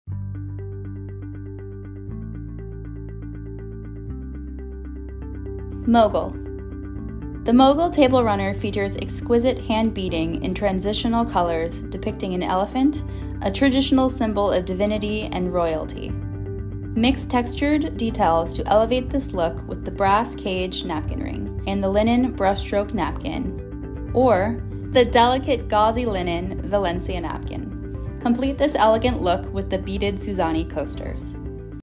Kim Seybert Spring 2021 Mogul Collection
The Mogul Table Runner features exquisite hand-beading
in transitional colors depicting an elephant, a traditional symbol of divinity
and royalty. Mix textured details to elevate this look with the brass Cage Napkin Ring and the linen Brushstroke Napkin or the delicate gauzy linen Valencia Napkin. Complete this elegant look with the beaded Suzani Coasters.

// ABOUT KIM SEYBERT //
Fashion for the Table. Kim Seybert designs tabletop decor and luxury home accessories inspired by travel, artisanship, and a love of entertaining.